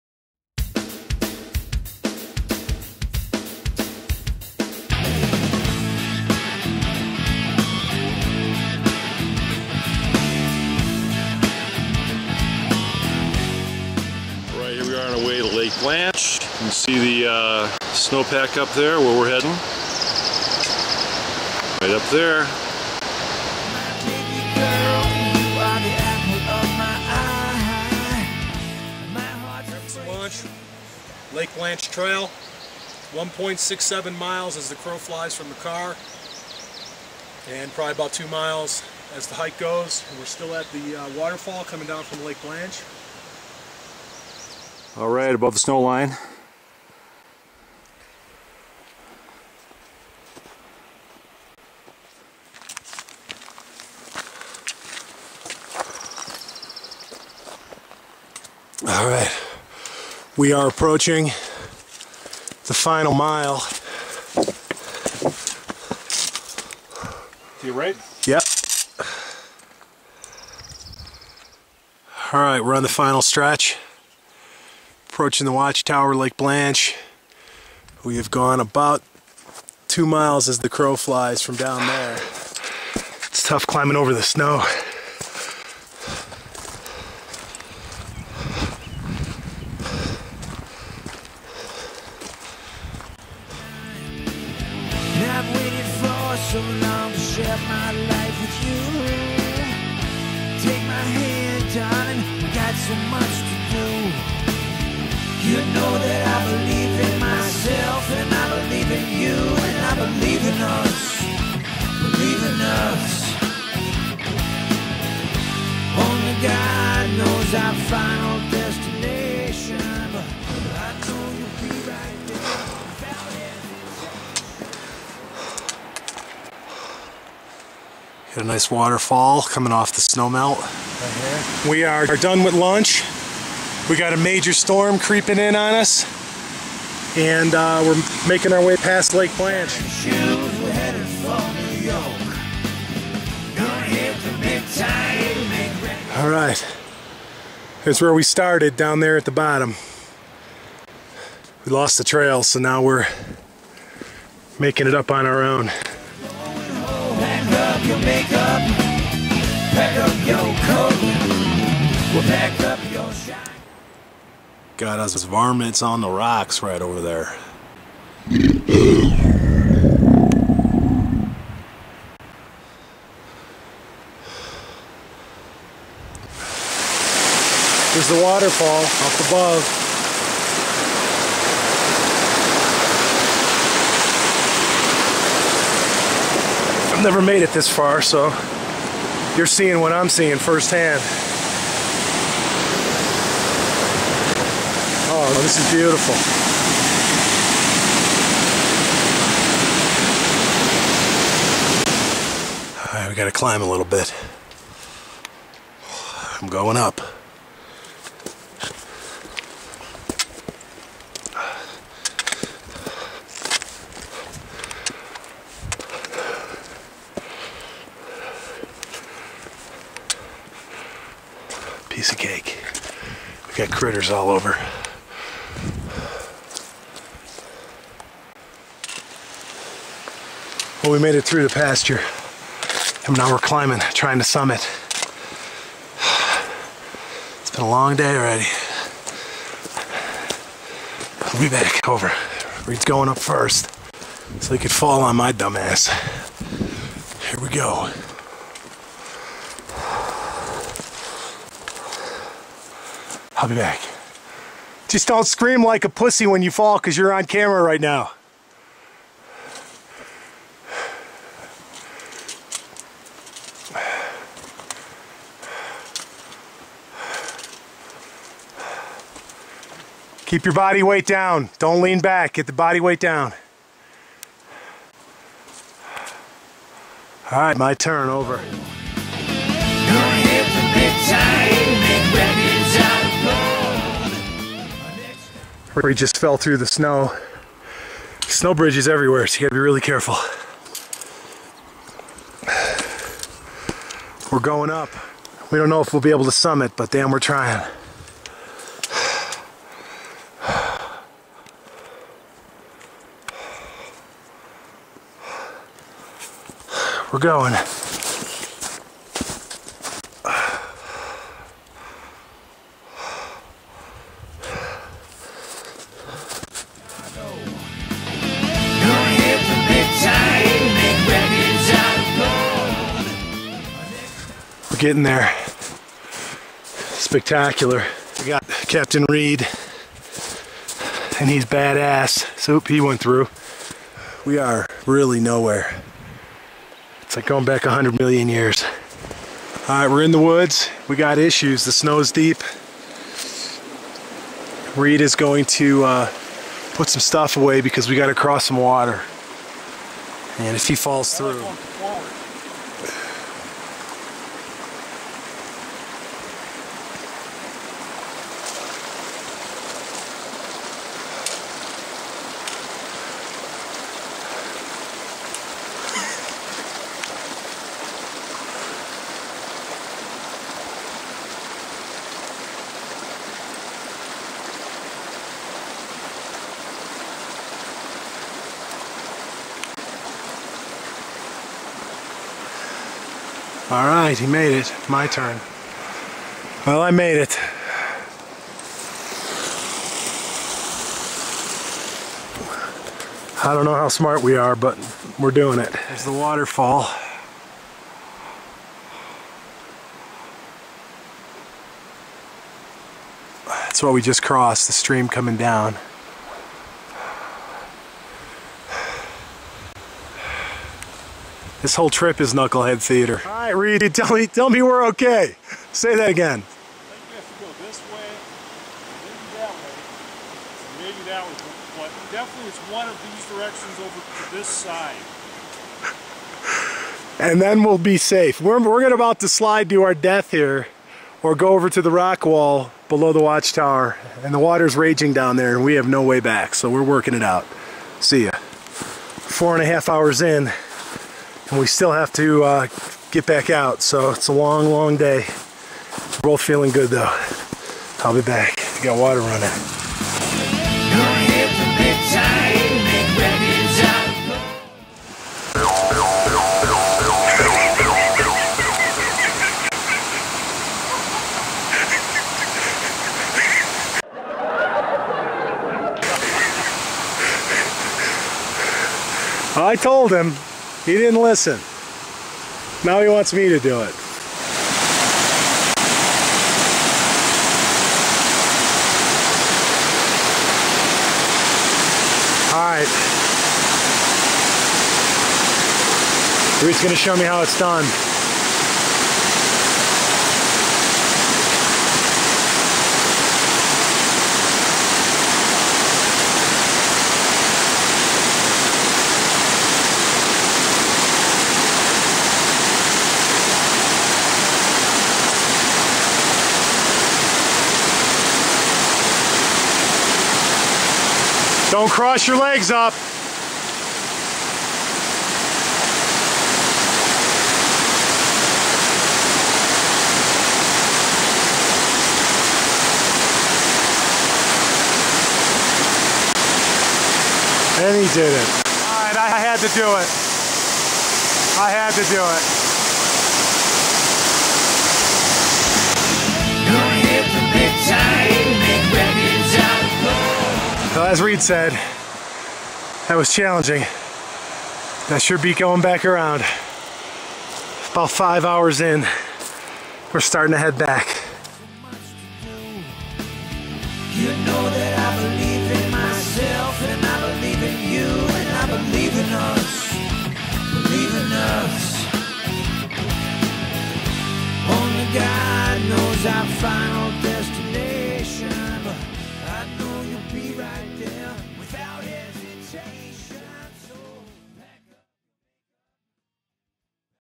LAKE BLANCHE, Big Cottonwood Canyon, Utah Spring 2009
Craig Paller and Reed Grothe are off on another adventure in the Wasatch Front.  Funny and beautiful scenery.  Mill B South trailhead, east of SLC.